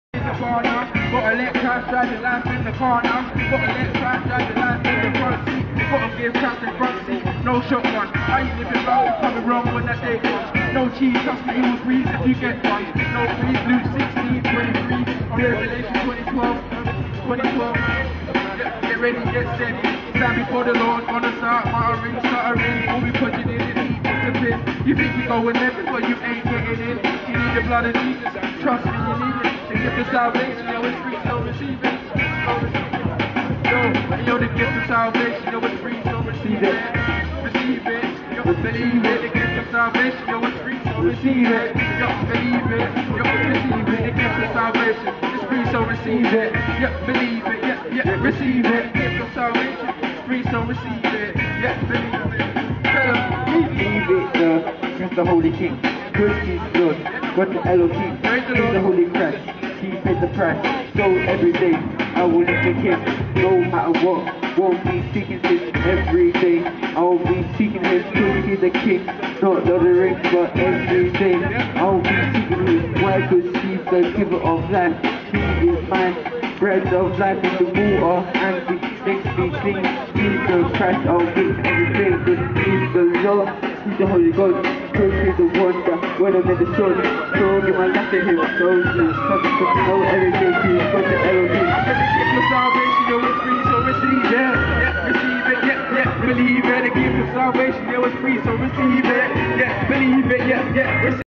Christian Rappers 1
These guys were in Brighton, doing some good freestyle rap about their beliefs.